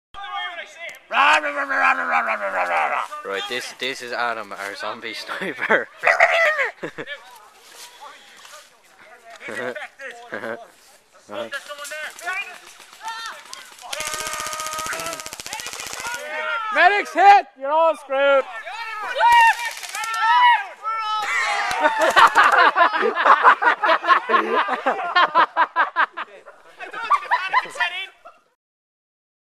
Outtakes #2 zombie noises and crying children
Some outtakes from the game on the 13-1-13 at The Manor Airsoft Site